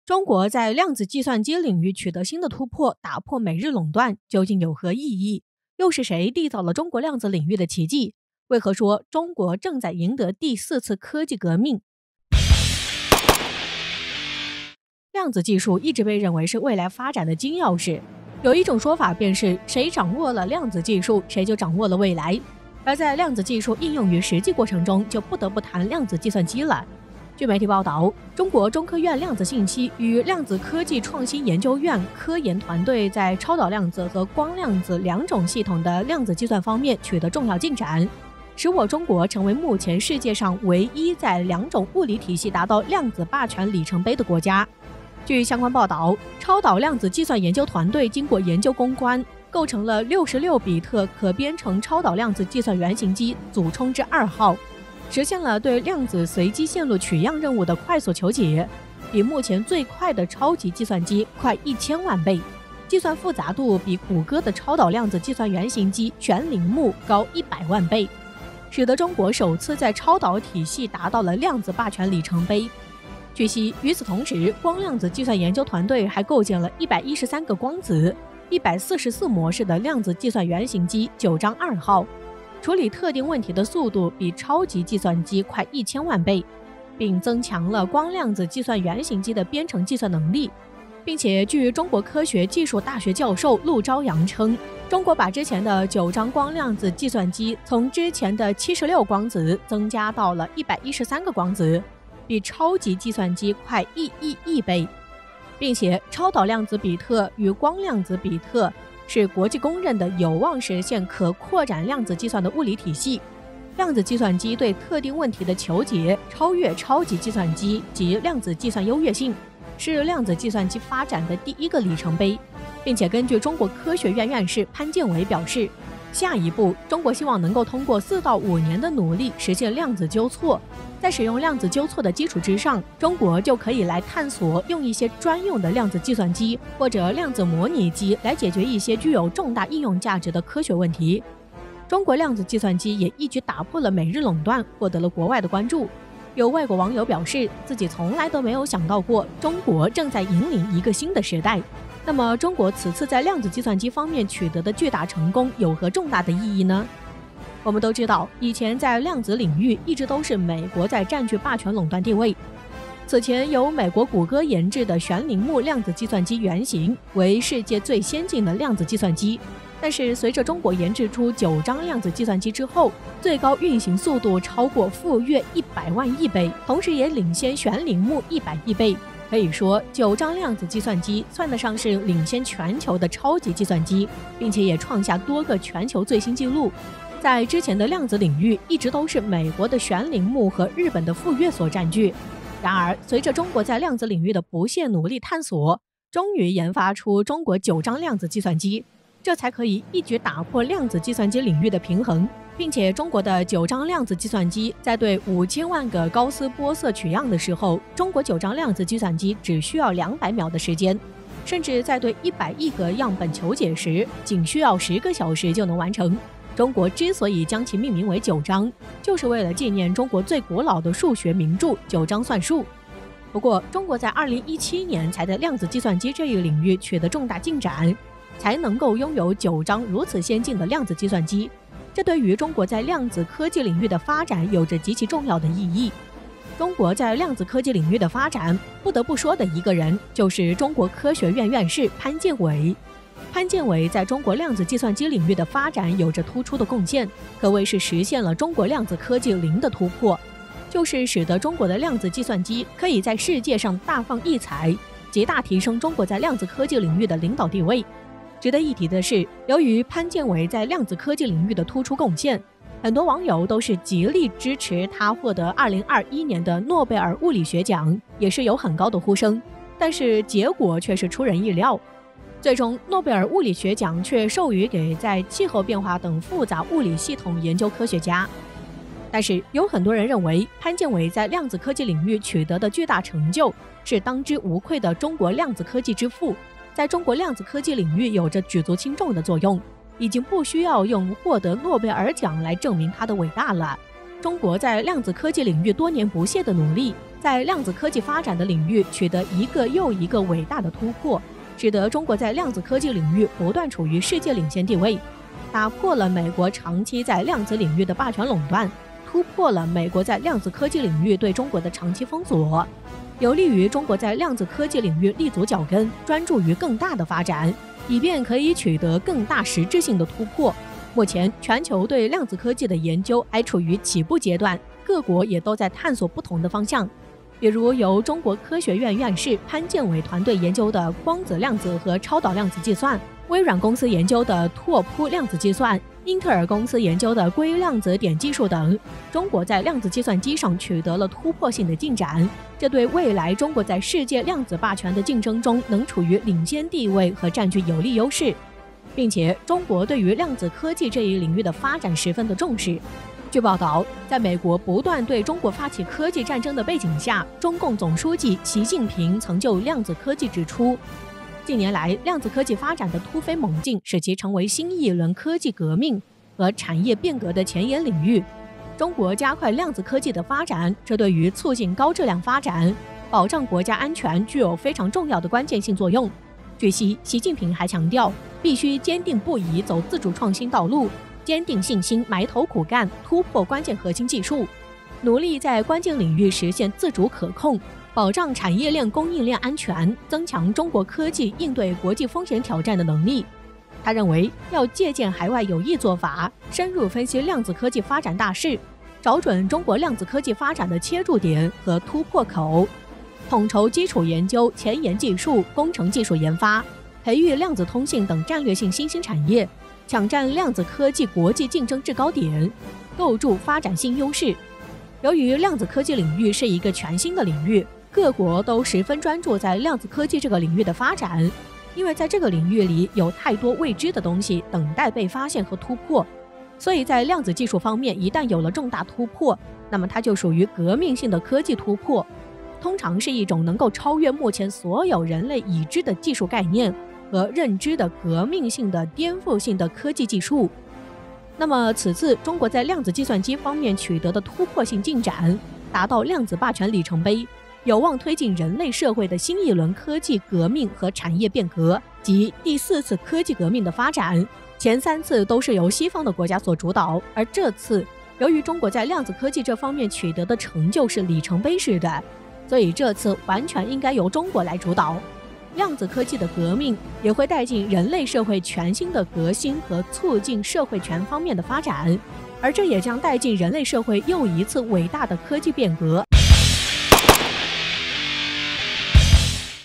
中國這一領域打破美日壟斷，贏得第四次科技革命，老外傻眼，網友：諾貝爾獎不給他我是不服的【一號哨所】
中國在量子計算機領域取得新的突破，打破美日壟斷，究竟有何意義？又是誰締造了中國量子領域的奇蹟？為何說中國正在贏得第四次科技革命
中國這一領域打破美日壟斷，贏得第四次科技革命，老外傻眼，網友：諾貝爾獎不給他我是不服的
一號哨所 軍事 量子領域 我們的影片會遵循公平、尊重的原則，確保不會包含任何仇恨言論的元素。